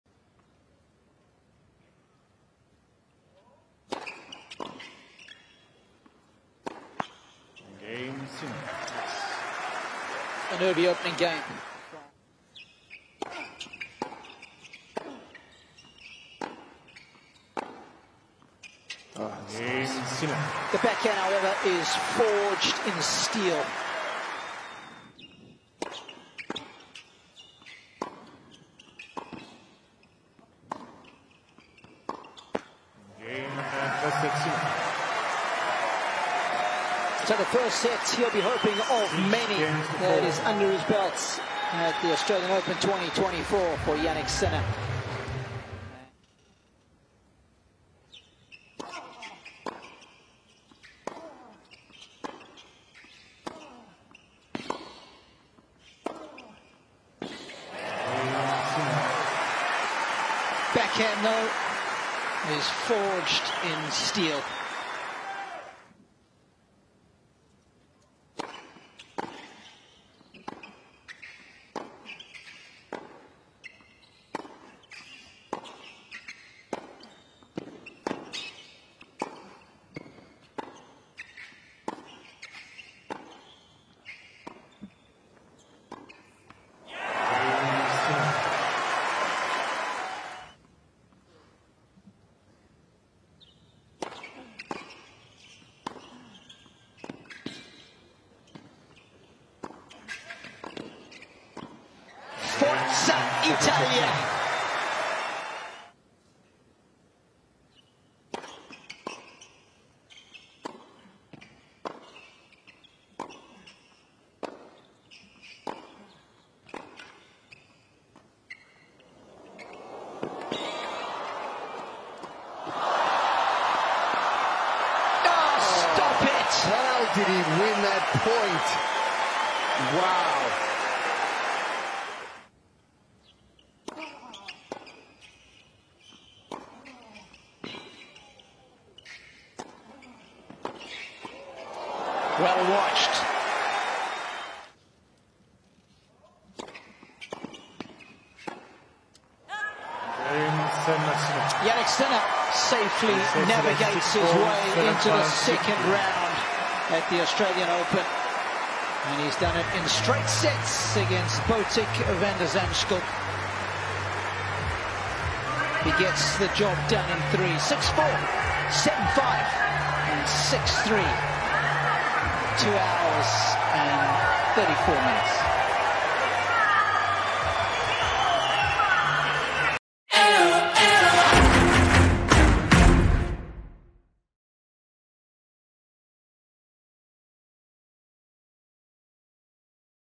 Jannik Sinner v Botic Van de Zandschulp Highlights | Australian Open 2024 First Round
Watch the highlights of Jannik Sinner v Botic Van de Zandschulp in the first round of the Australian Open 2024.